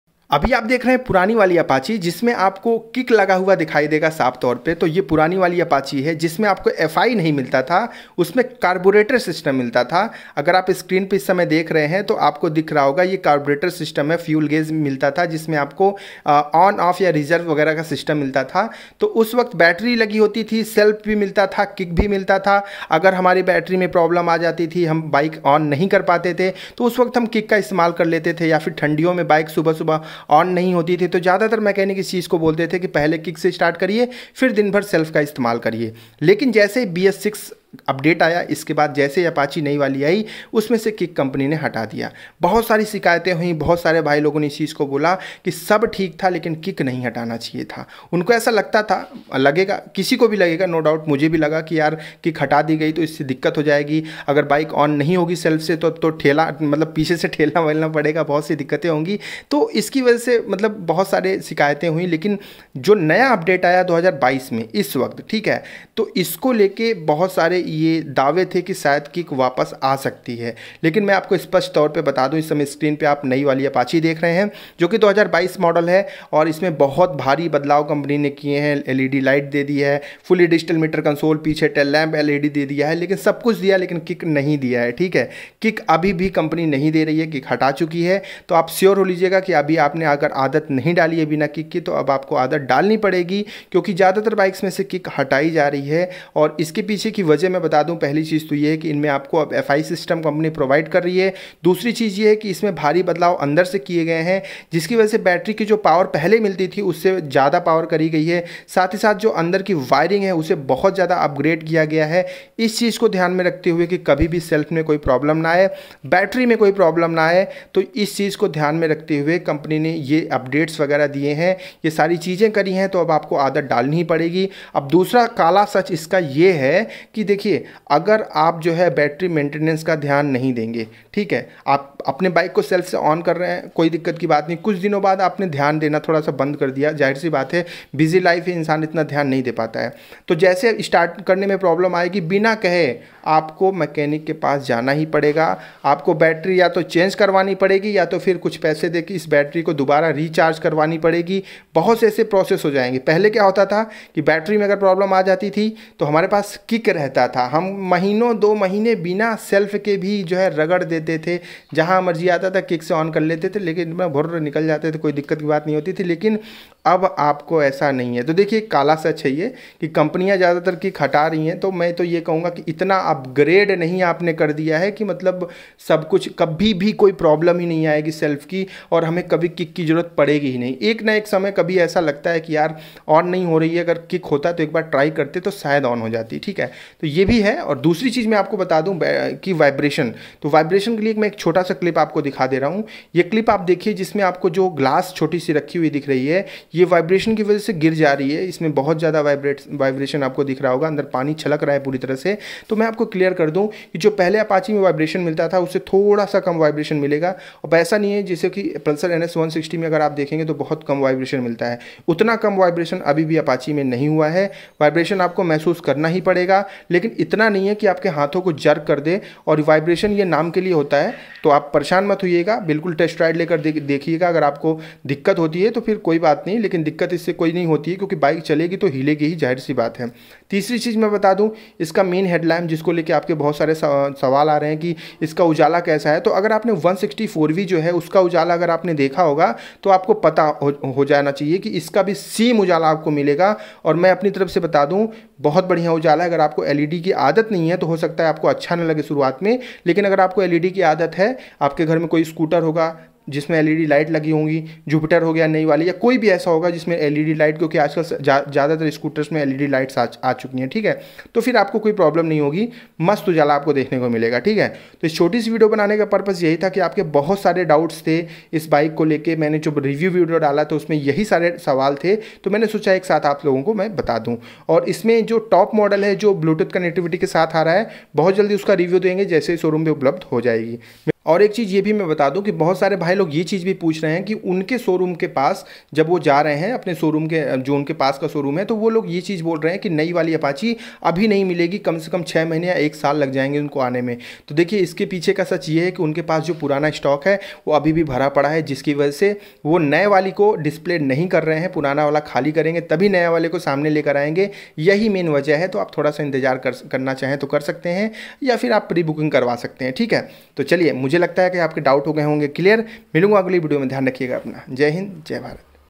2022 New TVS Apache rtr 160 Latest Update : Kick Start coming? Still Vibrating? Old Stock Apache?
Agar aap purani wali apache 160 2v lena chahte hai aur aapko ye janna hai ki kick kbtk aaega and vibration kya bhi tak hai to is video ko akhiri tk dekhiye !! aapke sabhi doubt clear ho jaege !!